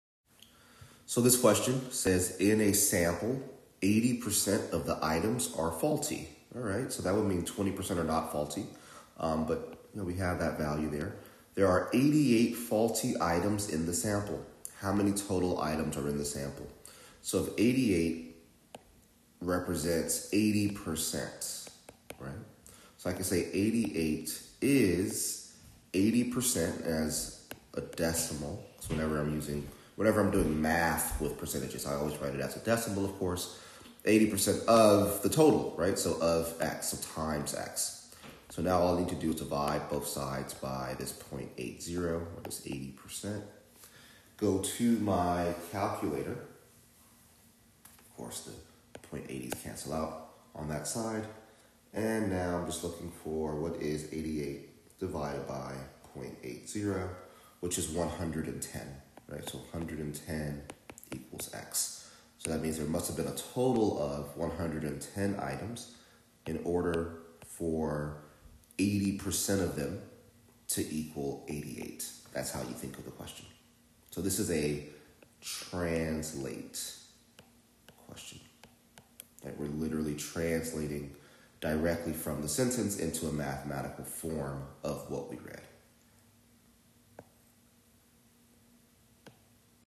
In a sample, 80% of the items are faulty. There are 88 faulty items in the sample. How many total...
Bluebook SAT Practice Test 7, Module 2 HARD, Question 3:

In a sample, 80% of the items are faulty. There are 88 faulty items in the sample. How many total items are in the sample?